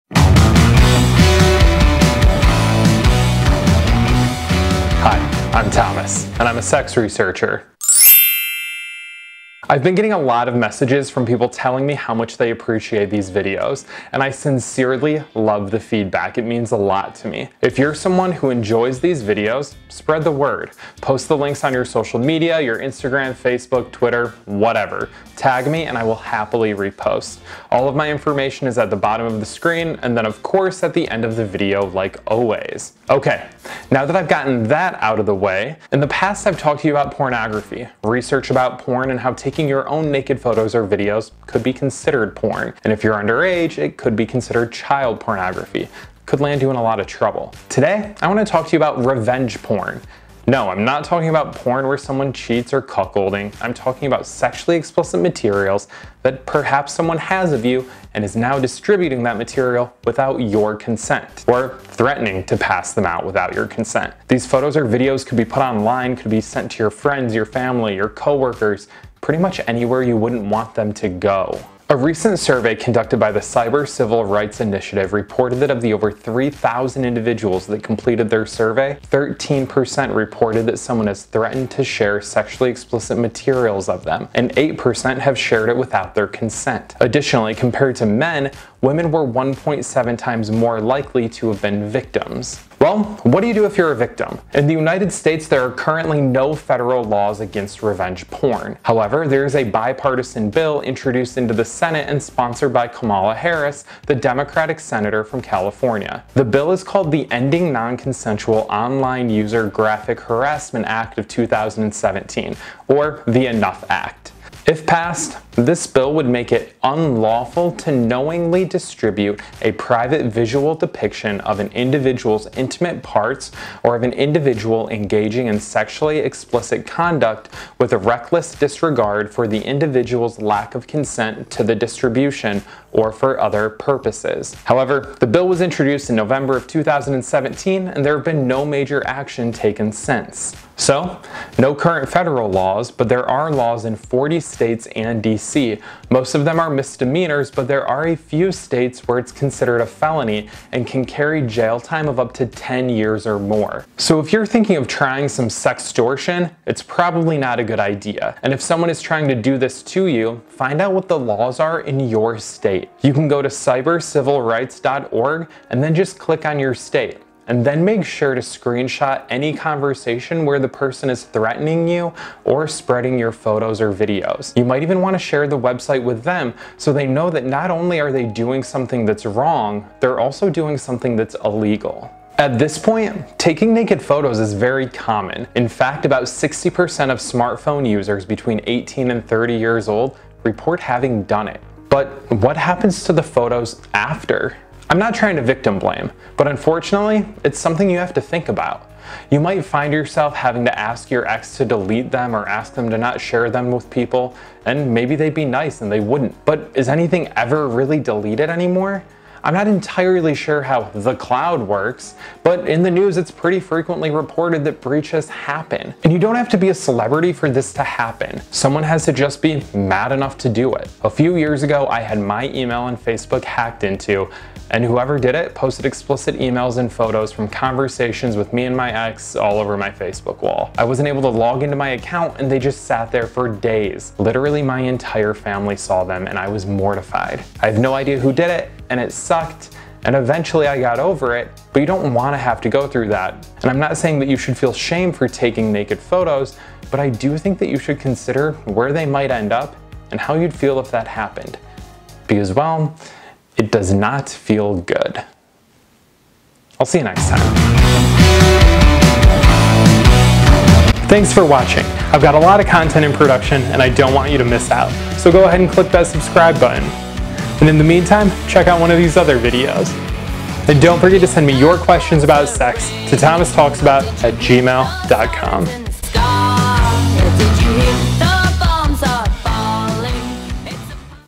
Revenge Porn
Episode 18: Revenge Porn

We've talked about porn a few times, and now we're back! This time, I'm taking on REVENGE PORN!!! Has anyone ever tried to use your sexually explicit photos or videos against you? A new national survey reveals that about 13% of people have had this happen to them. Did you know there are no federal laws to protect you? Kamala Harris has sponsored a bill, but it's been sitting waiting for over a year with nothing happening. A lot of states have passed legislation, but the penalty varies wildly.

How safe are your photos and videos? What would you do if something happened and everything was leaked online? I've had it happen. It wasn't fun.

SHIT THEY WON’T TELL YOU IN SEX ED